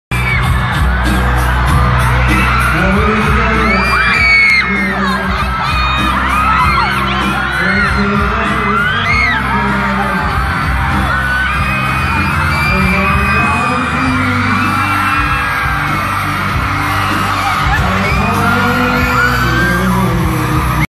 Jisoo manila fan meet
Jisoo went around the venue so this is a close up video